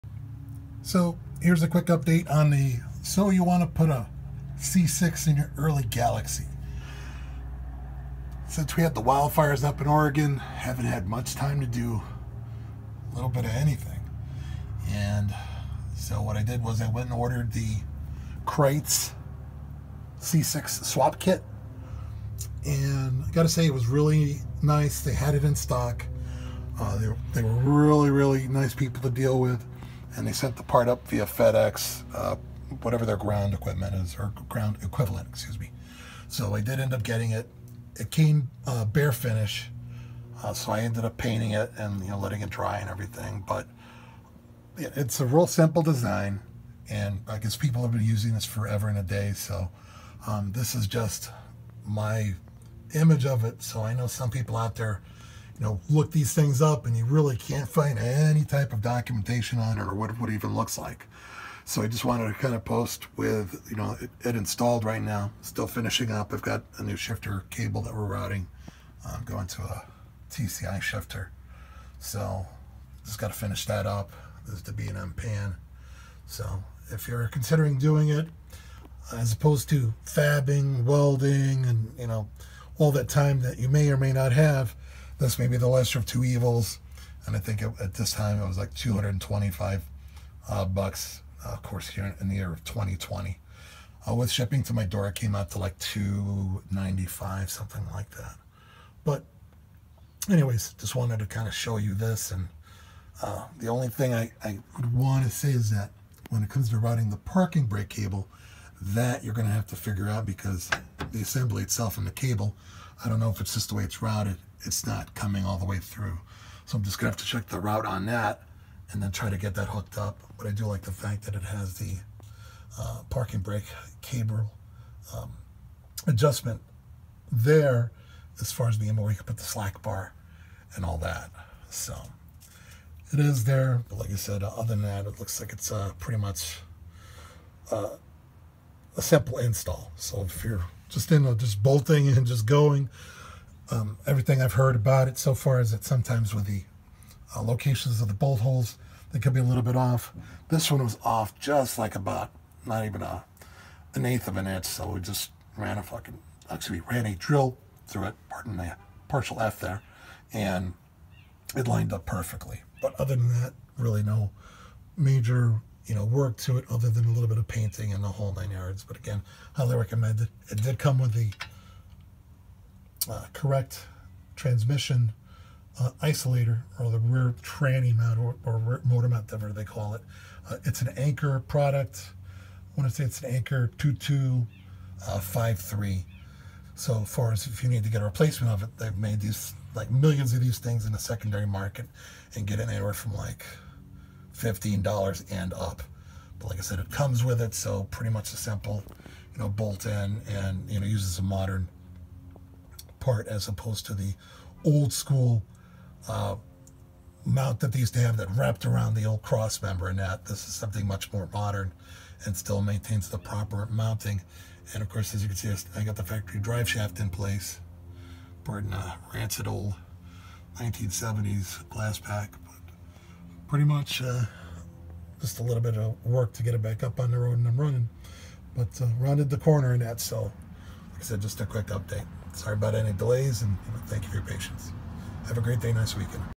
C6 Ford transmission swap using Crites Performance crossmember in a Ford Galaxie
Hey gang, here’s a quick update on the swap. The cost for the part has jumped to $325.00 as of 2/11/2022.

In my opinion it's money well spent and is the cornerstone of a successful transmission swap. If you can weld, you can save a bit of money but good luck with that.

It comes in bare metal finish and I recommend to paint it prior to installation.